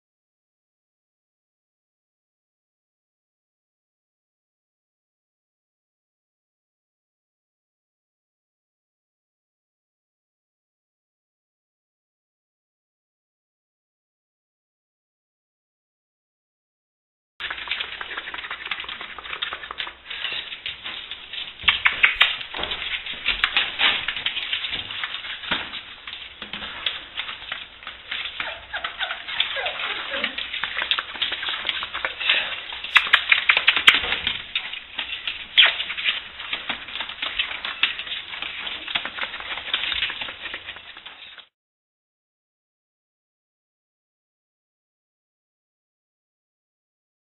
ERDEL TERIJER STENCI, VEGASKYWALKERS / AIREDALE TERRIER PUPS
Stenci naseg sampiona Lea i klupske prvakinje Astre, ostenjeni: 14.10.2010.
ODGAJIVACNICA ERDEL TERIJERA VEGA'S SKYWALKERS F.C.I.4243;
AIREDALE TERRIER KENNEL VEGA'S SKYWALKERS F.C.I.4243;